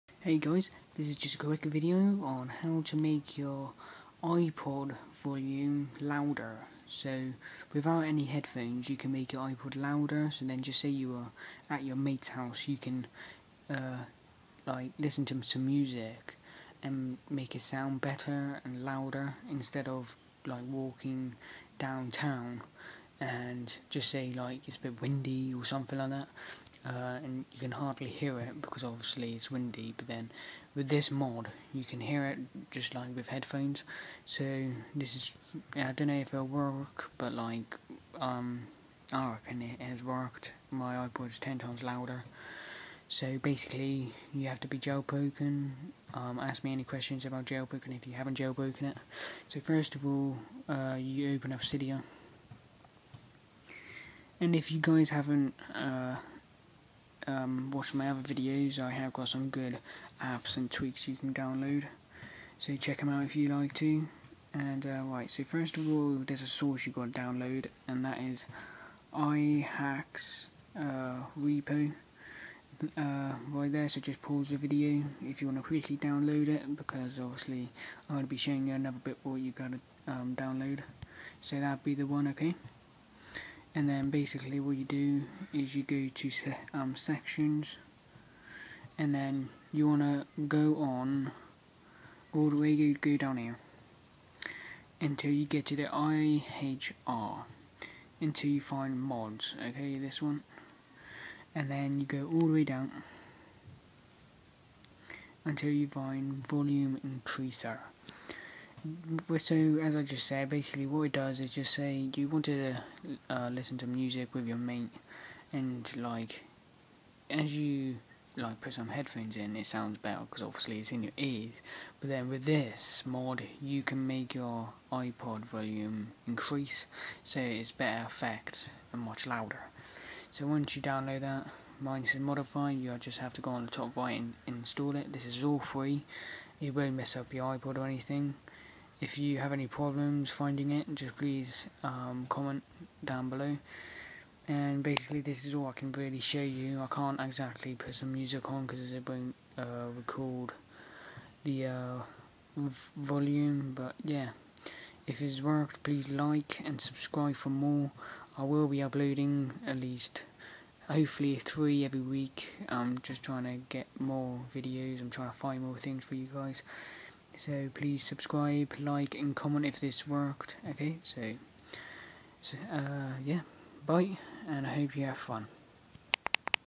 How to make your iPod volume louder iPod touch
Hey people this is how you make your iPod touch louder and better sub and like thanks